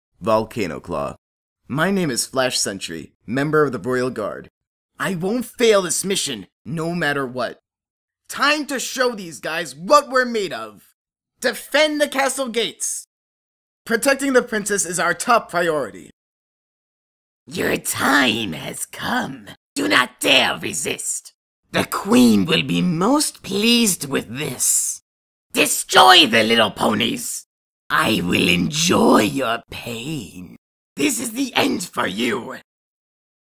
[Volcanoclaw] My Little Portal Audition- Flash Sentry and Changeling Soldier
Hello, Volcanoclaw here auditioning for ChristianCartoons' "My Little Portal" as Flash Sentry and Changeling Soldier. Enjoy.  :3